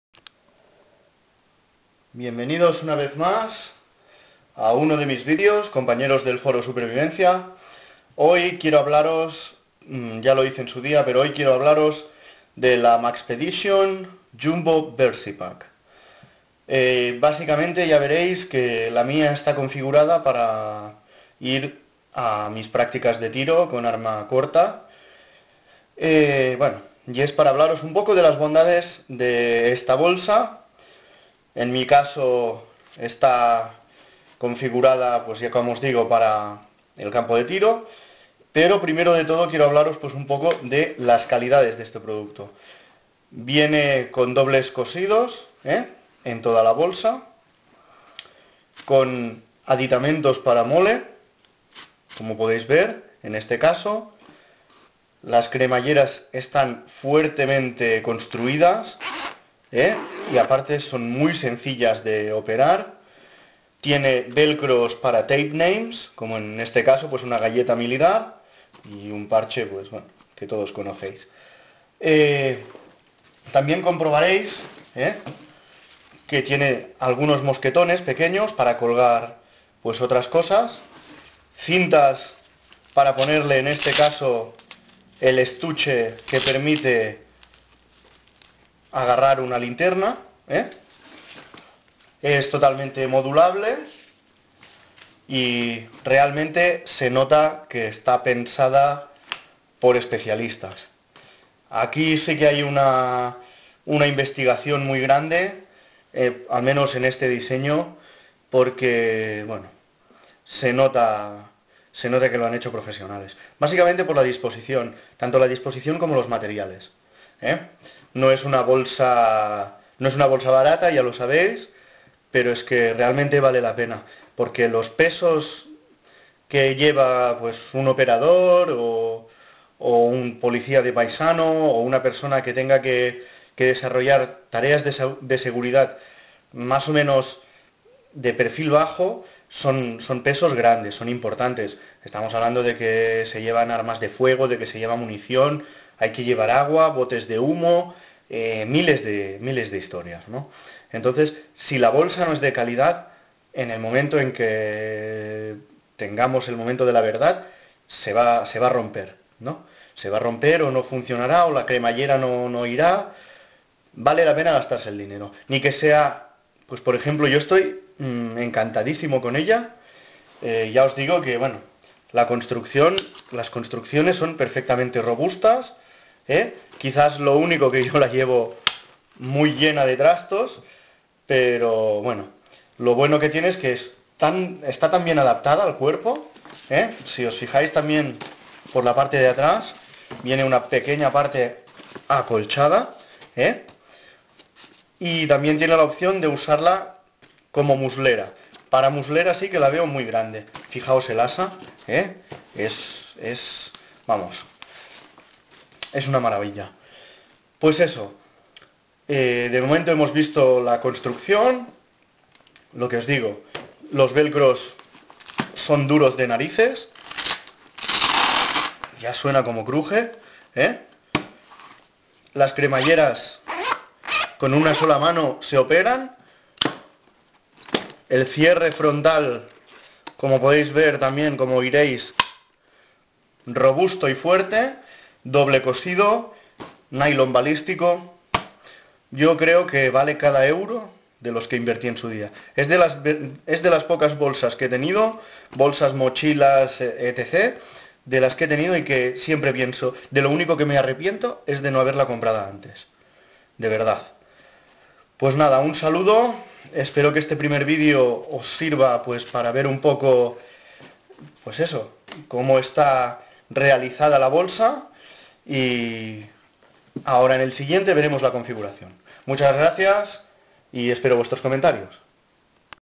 Maxpedition Jumbo Versipack Review Part I
Testeando una Maxpedition Jumbo Versipack para el Foro Todo Supervivencia  de España.
Testing a Maxpedition Jumbo Versipack for All  Survival  Forum from Spain.